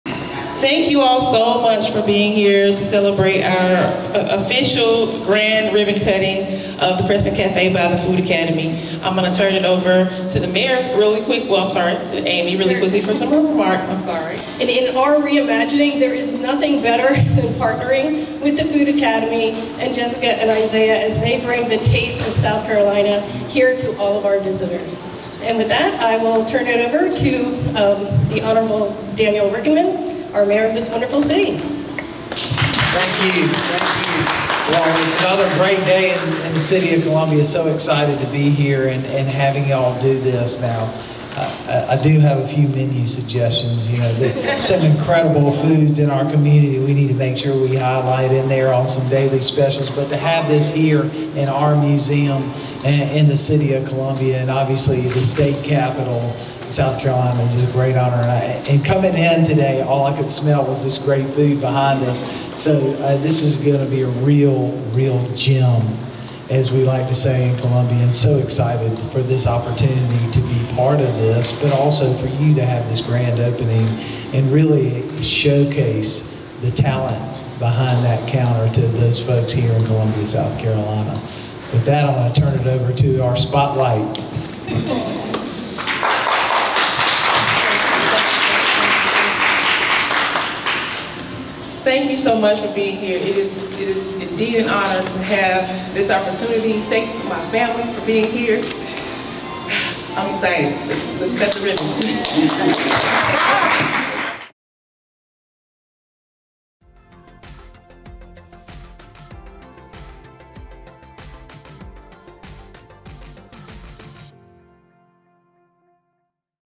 Crescent Café Grand Opening/Ribbon Cutting Ceremony
Mayor Daniel J. Rickenmann welcomed Crescent Café at a Grand Opening / Ribbon Cutting Ceremony.

Crescent Café by The Food Academy is located inside the South Carolina State Museum. Crescent Café will offer delicious selection of foods, snacks and sweet treats.

Visit Crescent Café at 301 Gervais Street.